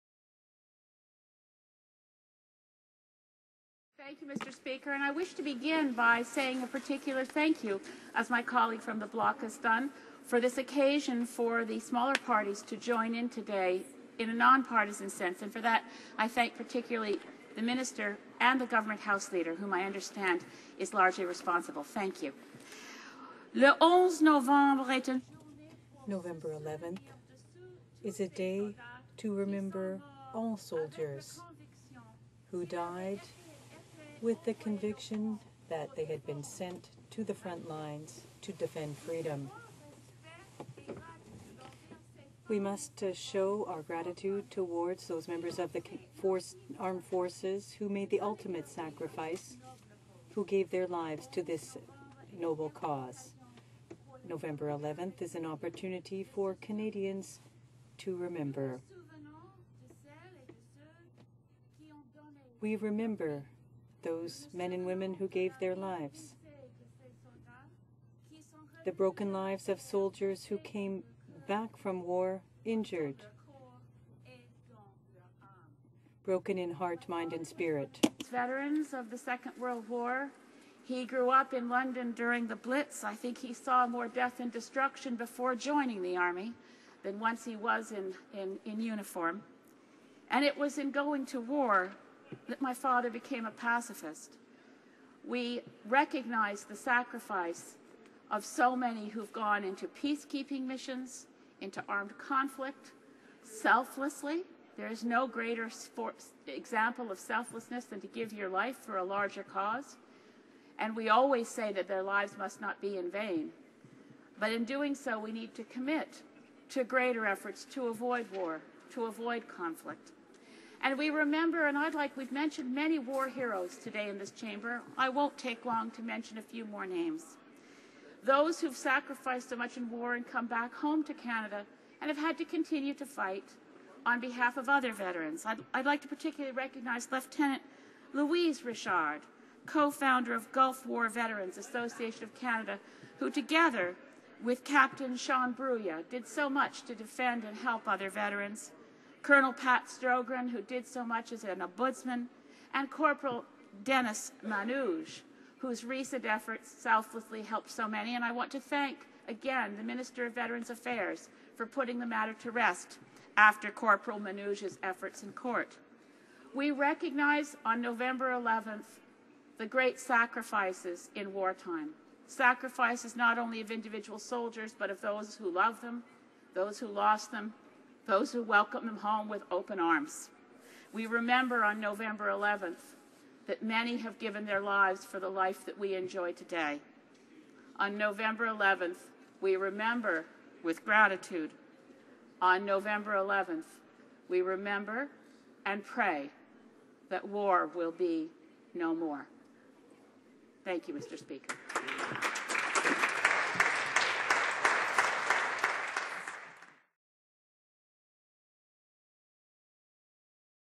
Elizabeth May: Tribute to Veterans
Elizabeth May: Mr. Speaker, I wish to begin by saying a particular thank you, as my colleague from the Bloc has done, for this occasion for the smaller parties to join in today in a non-partisan sense. For that I thank particularly the minister and the government House leader whom I understand is largely responsible.

November 11 is a day to remember all of the soldiers who died knowing that they had been sent to the front lines to protect our freedom. We owe our respect and gratitude to the men and women of the armed forces who made the ultimate sacrifice by giving their lives to this noble cause. November 11 is an opportunity for Canadians to remember.

We remember those who gave their lives. We remember the shattered lives of the soldiers who returned from the war wounded in body and spirit.

My father was one of the veterans of the Second World War. He grew up in London during the blitz. I think he saw more death and destruction before joining the army than once he was in uniform. It was in going to war that my father became a pacifist.

We recognize the sacrifice of so many who have gone into peacekeeping missions, into armed conflict, selflessly. There is no greater example of selflessness than people who give their lives for a larger cause and we always say that their lives must not be in vain, but in doing so, we need to commit to greater efforts to avoid war, to avoid conflict.

We have mentioned many war heroes today in this chamber. I will not take long to mention a few more names, those who have sacrificed so much in war, have come back home to Canada and have had to continue to fight on behalf of other veterans.

I would like to particularly recognize Lieutenant Louise Richard, co-founder of Gulf War Veterans Association of Canada who, together with Captain Sean Bruyea, did so much to defend and help other veterans; Colonel Pat Stogran, who did so much as an ombudsman; and Corporal Dennis Manuge, whose recent effort selflessly helped so many.

I want to thank again the Minister of Veterans Affairs for putting the matter to rest after Corporal Manuge's efforts in court.

We recognize on November 11 the great sacrifices in war time, sacrifices not only of individual soldiers, but of those who love them, those who lost them, those who welcome them home with open arms. We remember on November 11 that many have given their lives for the life that we enjoy today.

On November 11, we remember with gratitude. On November 11, we remember and pray that war will be no more.